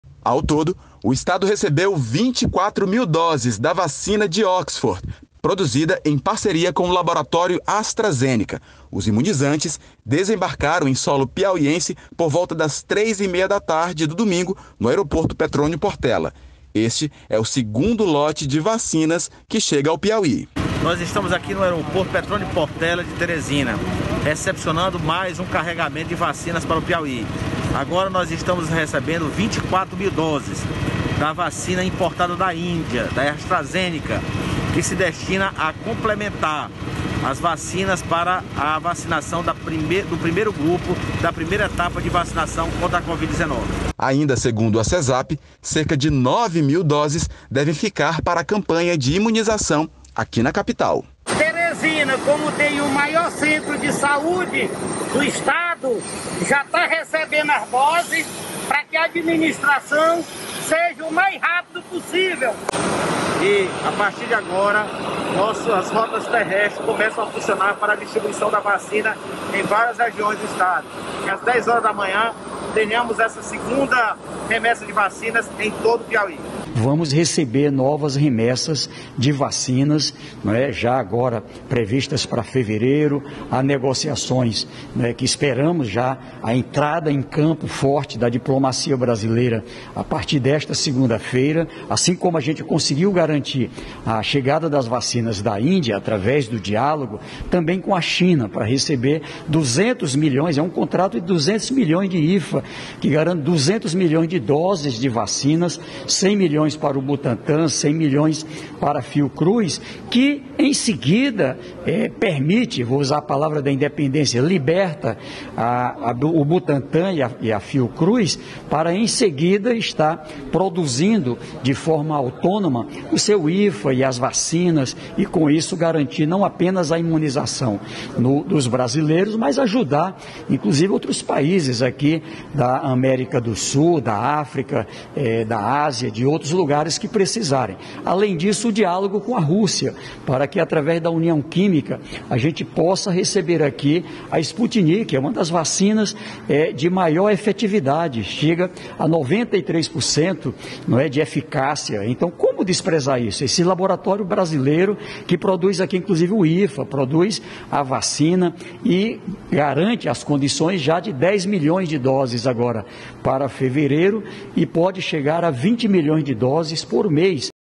Mais doses da vacina contra a covid-19 chega ao Piauí
O Piauí recebe mais doses da vacina contra a covid-19. Com a chegada desse lote o estado deve alcançar 62% do total do público alvo na primeira etapa de vacinação.  A reportagem é de André dos Santos, da TV Assembleia.